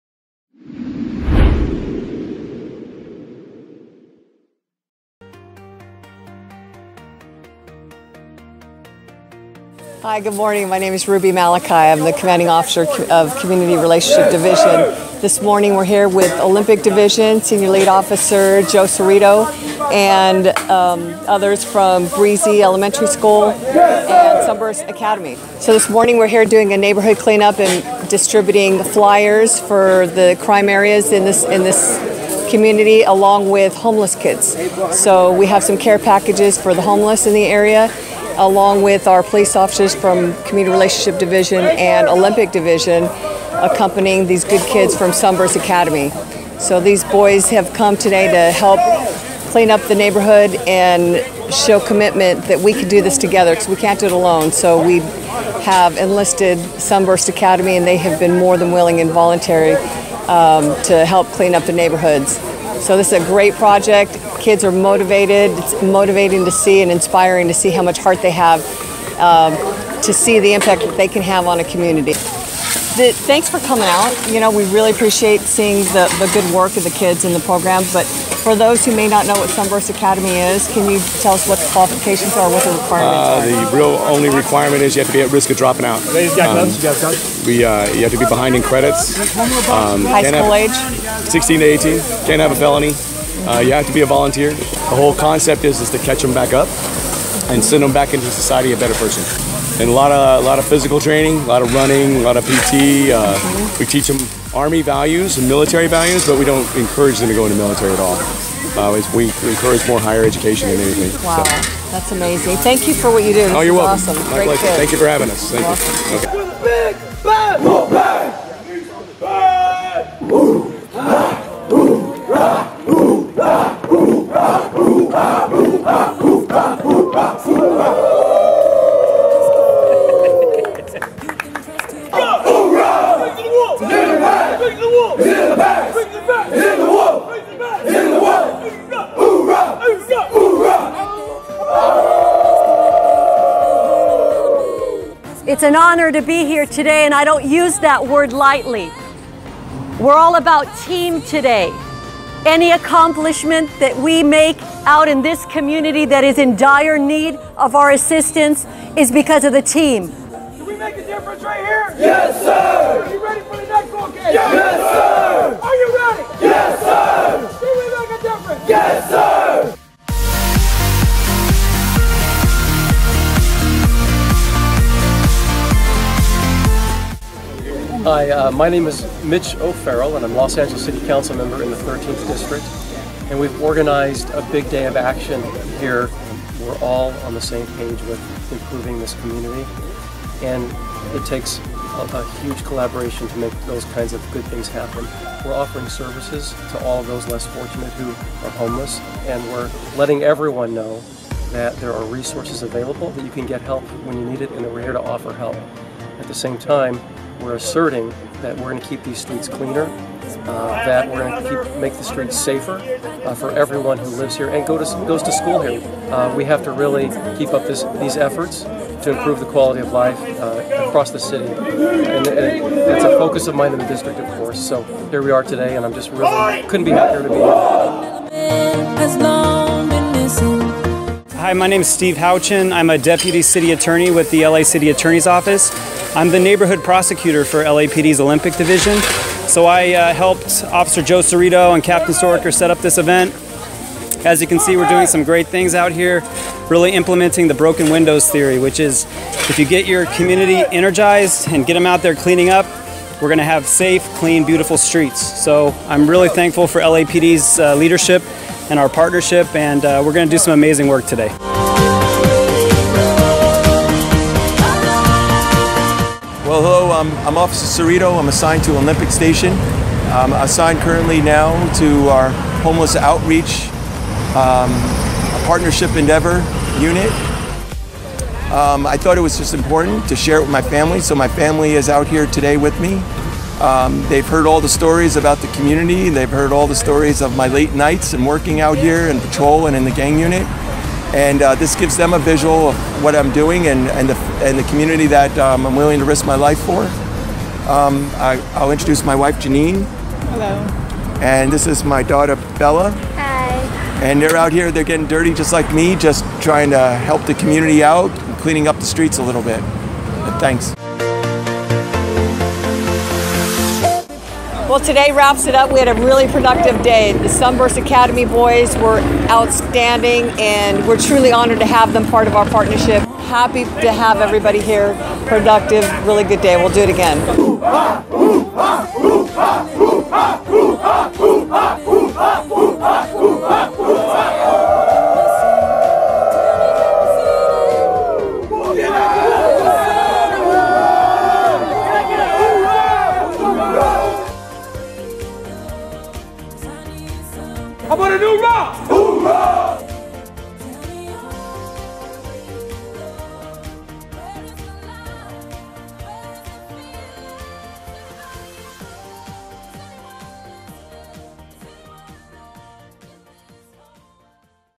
Sunburst Youth Academy with the LAPD Olympic Divison
Olympic Division partnered up with the Sunburst Youth Academy to host a homeless outreach event that included City Council member Mitch O'Farrell, Deputy Chief Beatrice Girmala, Captain Ruby Malachi, Captain David Storaker, the Sunburst Youth Academy volunteers, Olympic and Community Relationship Division personnel and local community members pitched in to help clean up the streets near 3rd and Vermont. The event was coordinated by Officer Joe Cirrito of Olympic Division on August 22, 2015.